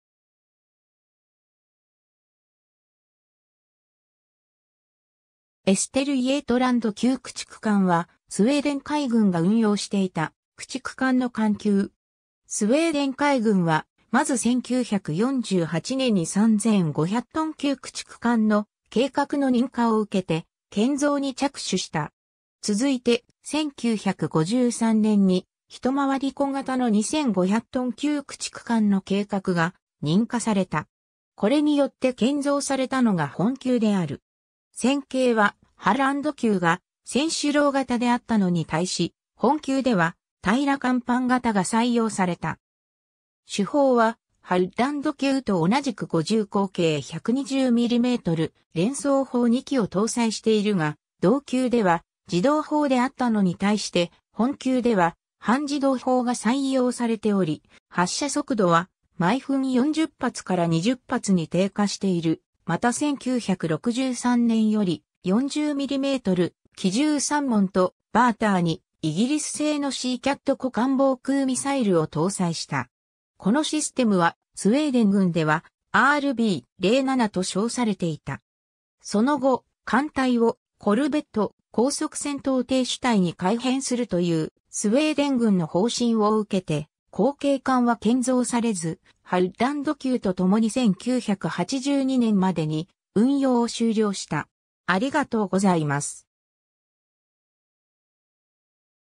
エステルイェートランド級駆逐艦
エステルイェートランド級駆逐艦（エステルイェートランドきゅうくちくかん、スウェーデン語: Jagare typ Östergötland; Östergötlandsklass）は、スウェーデン海軍が運用していた駆逐艦の艦級。
 スウェーデン海軍は、まず1948年に3,500トン級駆逐艦（ハッランド級）の計画の認可を受けて建造に着手した。
続いて、1953年に、ひとまわり小型の2,500トン級駆逐艦の計画が認可された。
これによって建造されたのが本級である。
船型は、ハッランド級が船首楼型であったのに対し、本級では平甲板型が採用された。
 主砲はハッランド級と同じく50口径120mm連装砲2基を搭載しているが、同級では自動砲であったのに対して本級では半自動砲が採用されており、発射速度は、毎分40発から20発に低下している、また1963年より、40mm機銃3門とバーターに、イギリス製のシーキャット個艦防空ミサイルを搭載した。
このシステムは、スウェーデン軍ではRb.07と称されていた。
 その後、艦隊をコルベット・高速戦闘艇主体に改編するというスウェーデン軍の方針を受けて、後継艦は建造されず、ハッランド級とともに1982年までに運用を終了した。